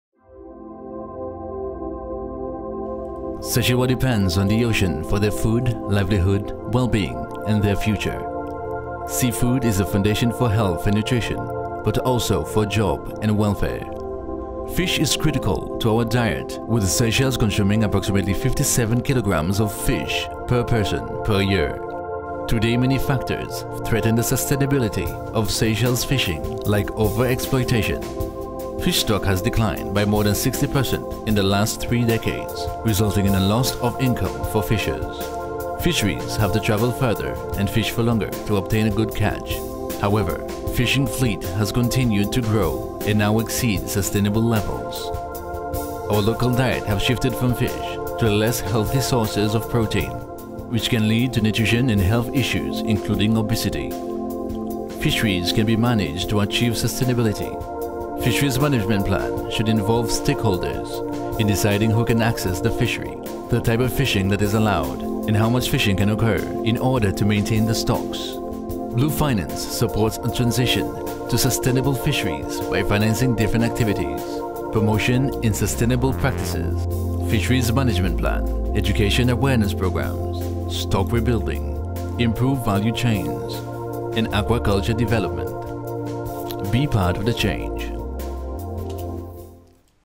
Sustainable Fisheries in Seychelles
The Seychelles is facing increasing evidence that the ocean’s natural capital is being depleted as a result of unsustainable economic activity including fisheries.
A transition to sustainable fisheries is vital for ensuring that we continue to enjoy social, economic and cultural benefits from our natural capital.
Sustainable fisheries will provide for our present and future needs, while also conserving fish populations and their habitats and marine ecosystems. Securing the rights of fishers to access fisheries resources is a key element in achieving sustainable fisheries.